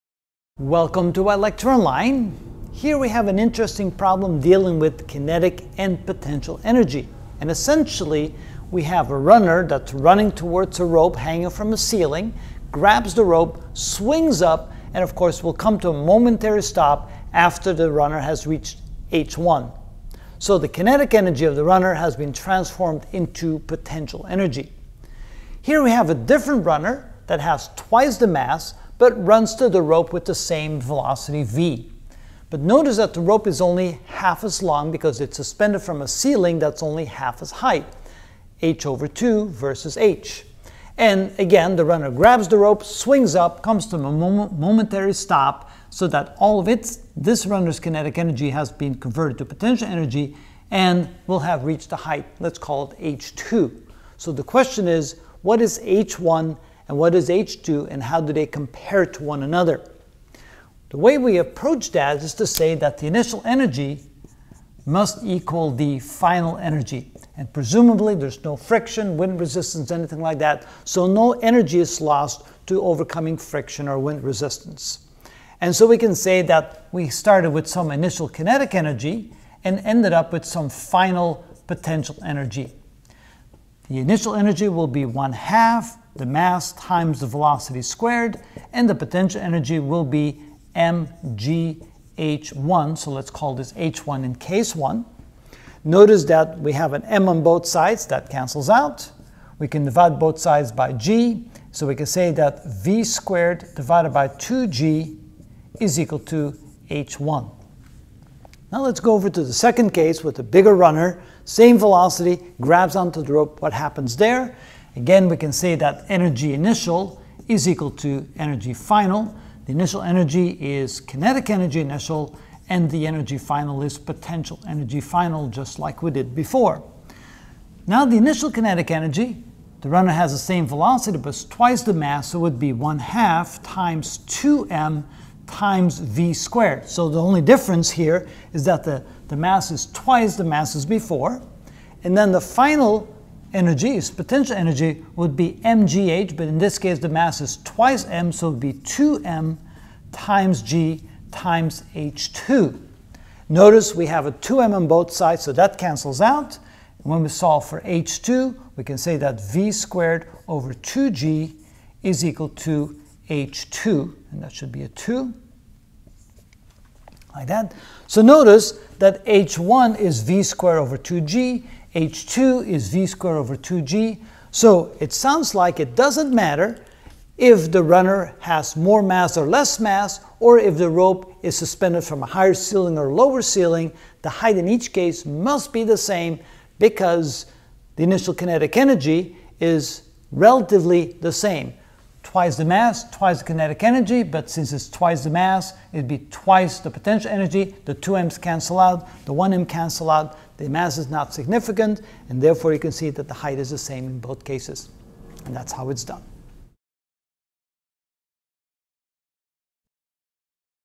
Physics 8.1 Work, Energy, and Power Examples (5 of 27) Change in Potential Energy
In this video I will explain kinetic and potential energy with 2 cases where a runner running towards with velocity=v, jumping on, and swing on a rope of length H and H/2, respectively, and finding the maximum heights of each swing.